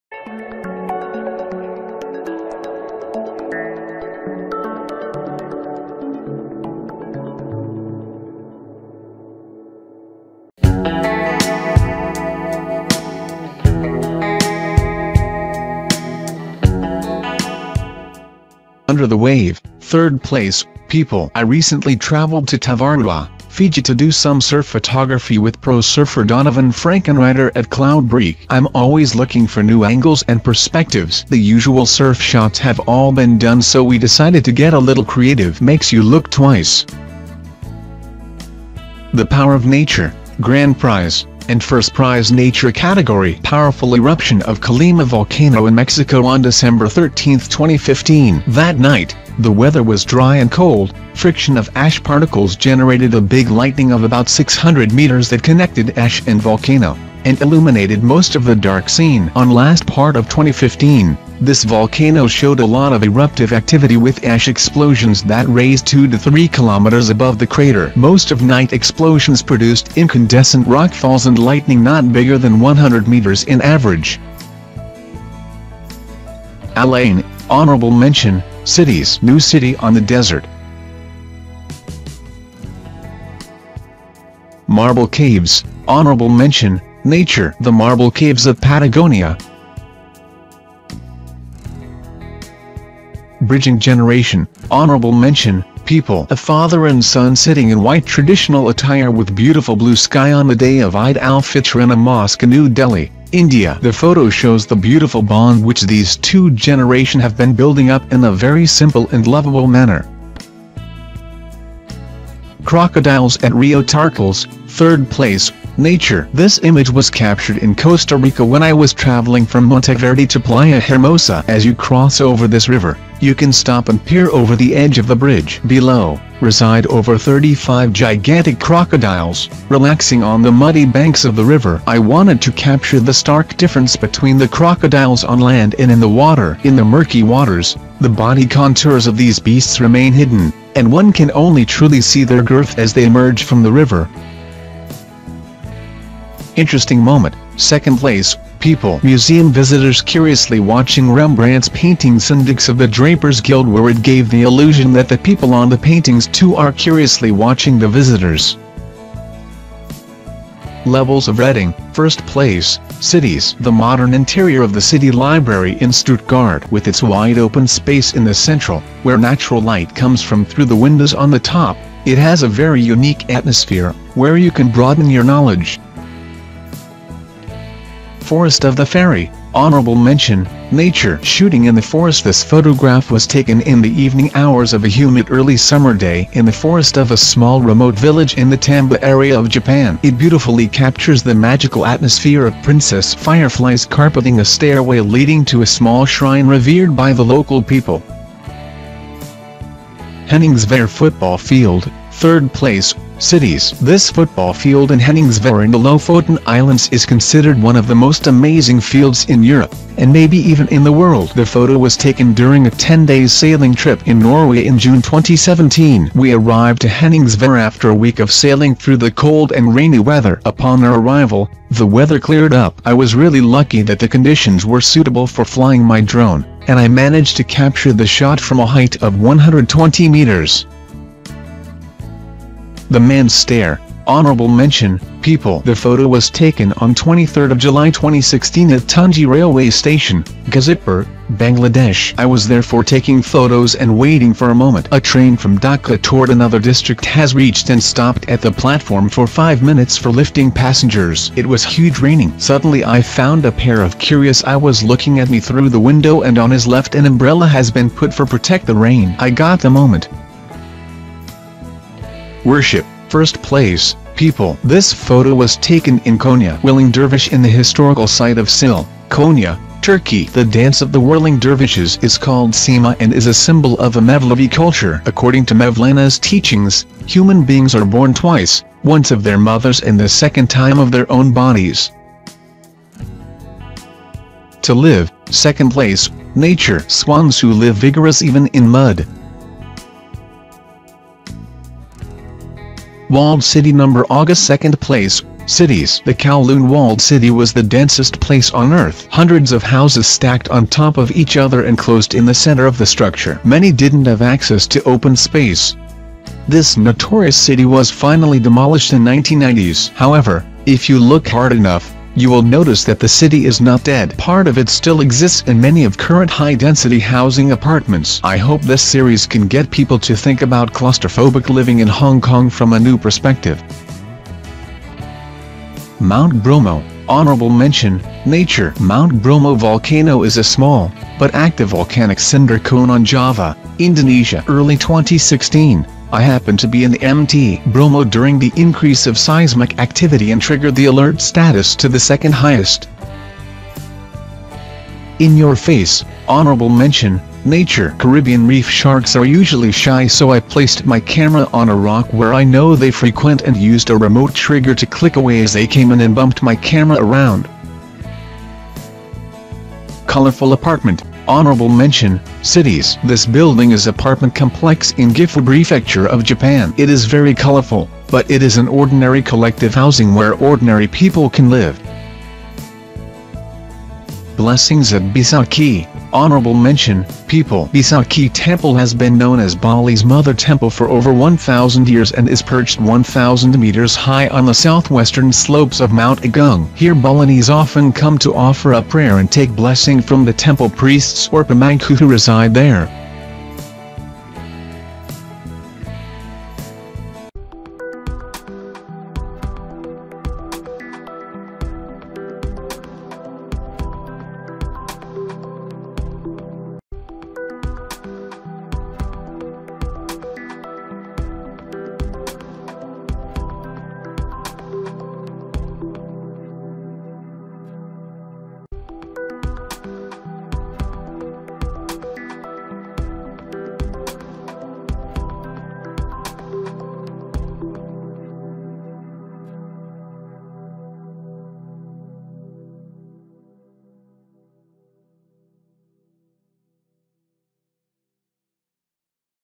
Photo Contest 2017: Winners of the 2017 National Geographic Travel Photographer of the Year Contest
The winners of the 2017 National Geographic Travel Photographer of the Year have been announced. And, as always, the photographs are spectacular.
Sergio Tapiro Velasco of Mexico was awarded the prestigious grand prize for his awe-inspiring image of the moment an erupting volcano was hit by a bolt of lightning.
National Geographic also awarded prizes for photographs in three categories -- Nature, Cities and People. Velasco's image snagged first prize in the Nature category as well.
Norbert Fritz of Hungary took top prize in the Cities category for his photo titled "Levels of Reading," while "Worship" by F. Dilek Uyar of Turkey won the People category.
The prize-winning photos, as well as several honorable mentions, are in the video.